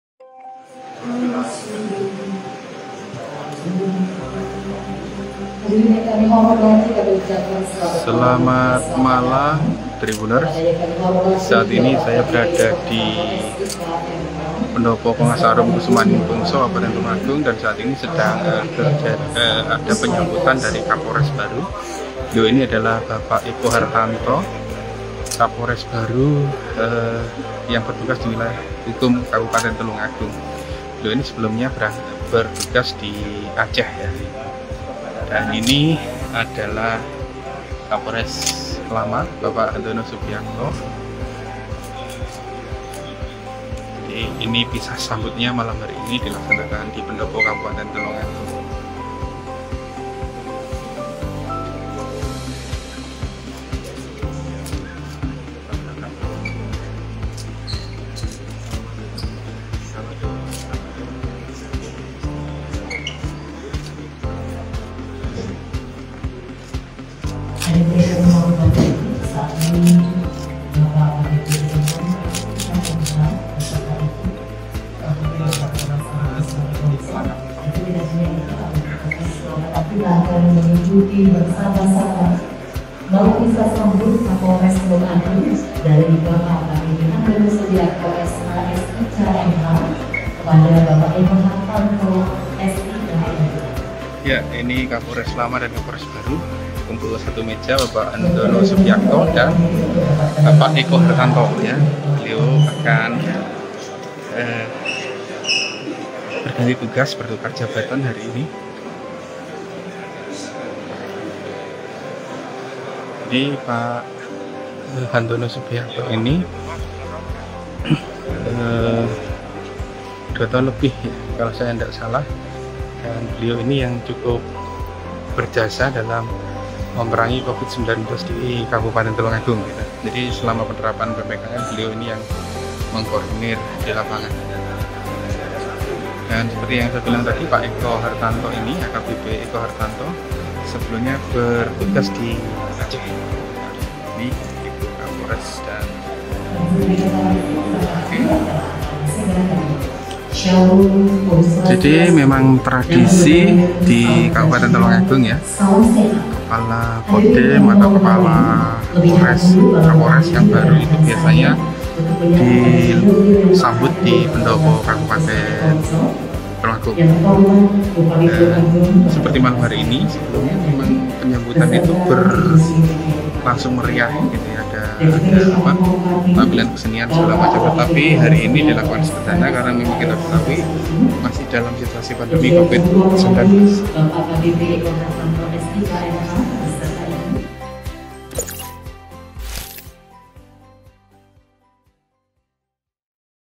Pisah Sambut Kapolres Tulungagung dari AKBP Handono Subiakto Kepada AKBP Eko Hartanto
TRIBUNMATARAMAN.COM | TULUNGAGUNG - Pisah sambut Kapolres Tulungagung dari AKBP Handono Subiakto kepada AKBP Eko Hartanto, Selasa (12/7/2022) malam di Pendopo Kongas Arum Kusumaning Bongso Kabupaten Tulungagung. Keduanya telah melakukan serah terima jabatan di Polda Jawa Timur.